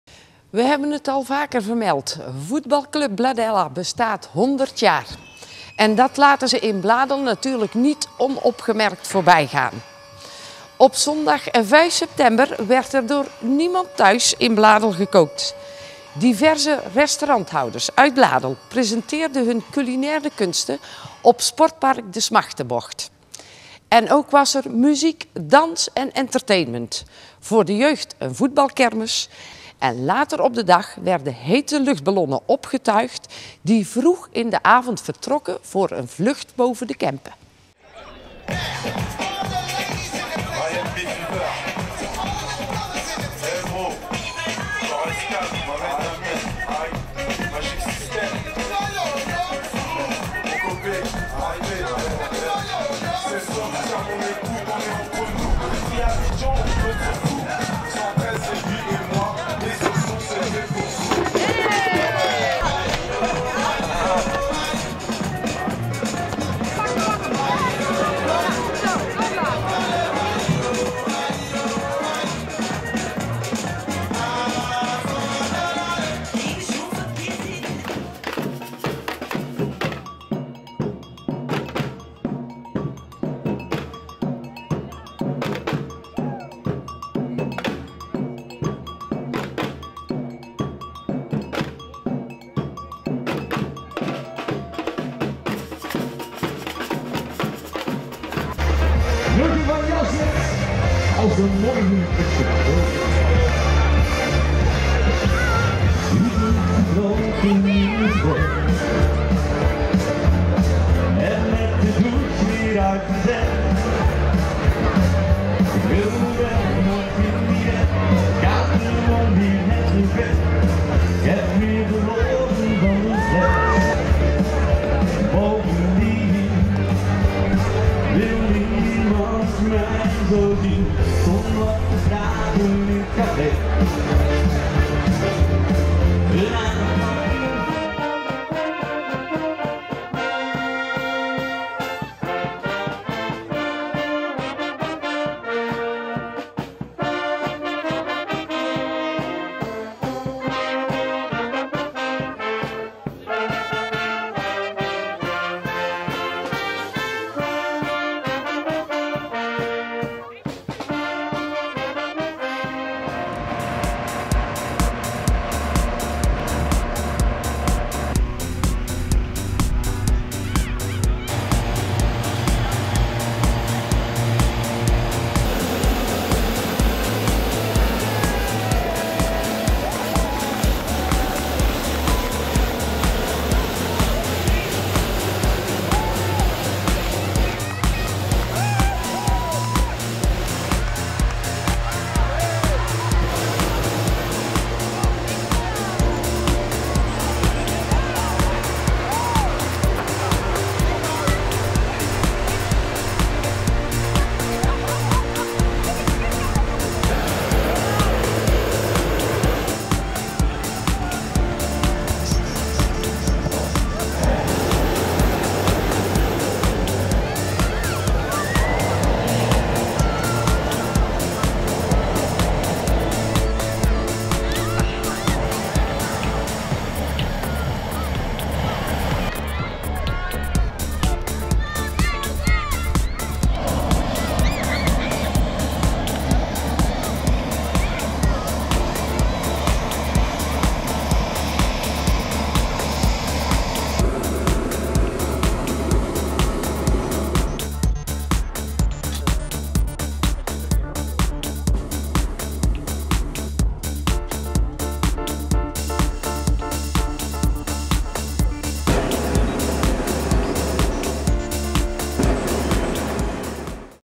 2010 september Bladel Bladella Culinair
kempen tv uitzending 2010 september Bladella Culinair,ook was er muziek,dans en ander entertainment,voor de jeugd was er een voetbalkermis,
en er werden hete luchtballonnen opgetuigd voor een vlucht boven de kempen.